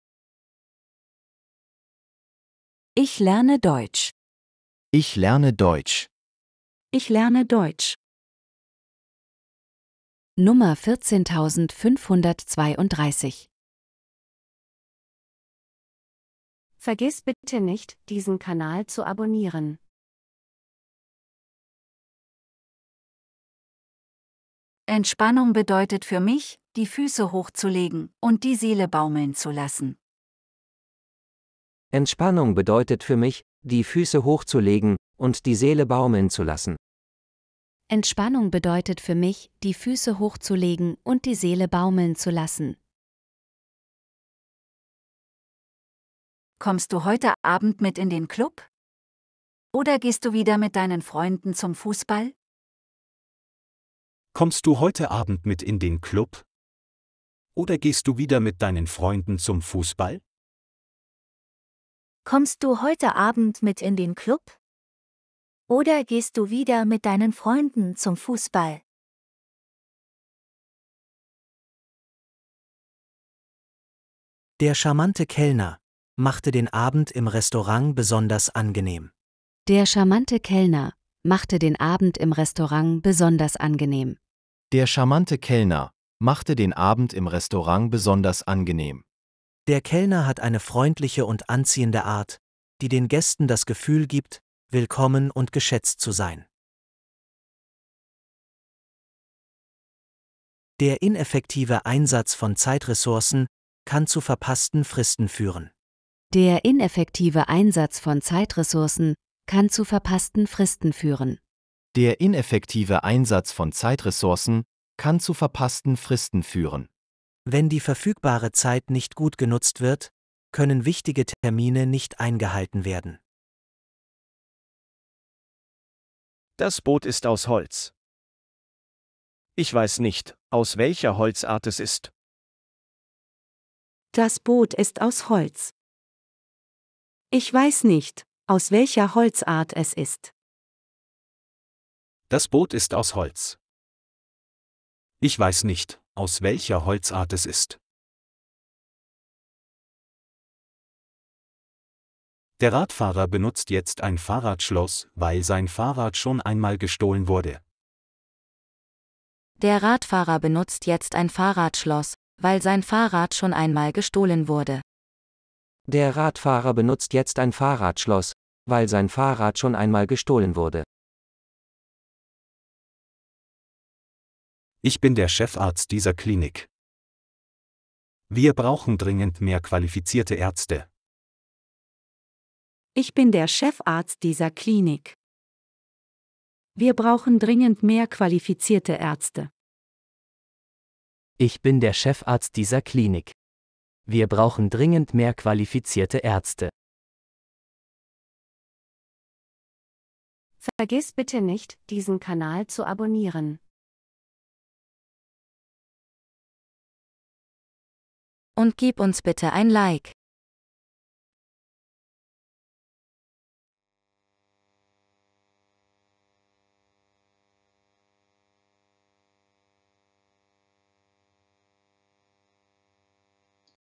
▶️14532▶️Ich lerne Deutsch mit Texten und Bildern in unterschiedlichen Situationen.
A
✅aber✅abgehalftert✅abgeriegelt✅abgeschlossen✅abweisend✅allein✅allergisch

L
✅lediglich✅legendär✅legitim✅lehrreich✅limitiert✅lückenhaft✅luftig✅luxuriös

W
✅weitgehend✅wenig✅weniger als✅wissenschaftlich✅wirtschaftlich